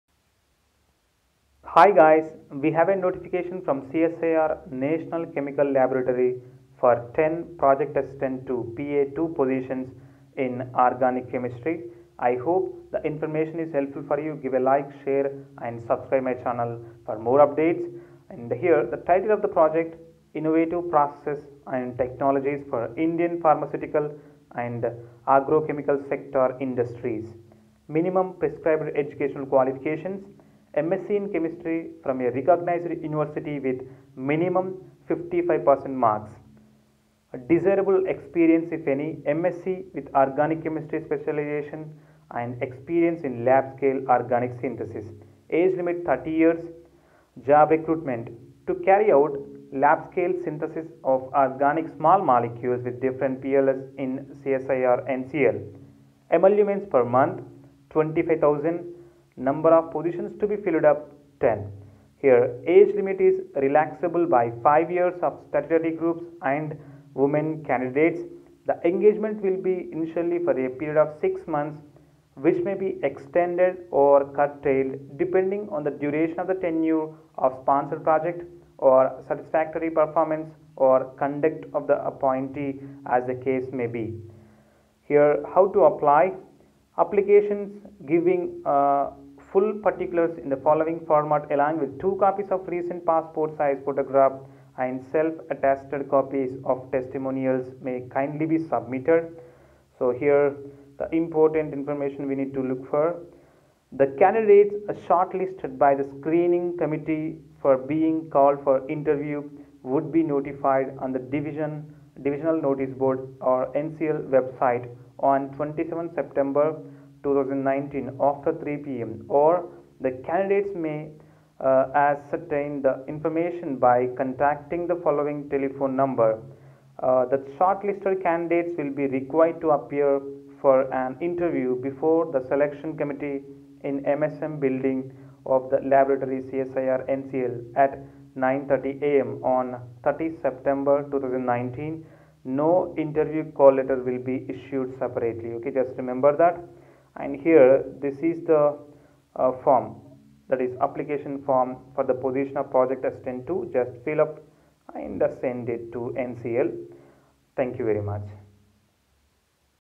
CSIR NCL project assistant -II jobs
csir NCL project assistant -II positions in organic chemistry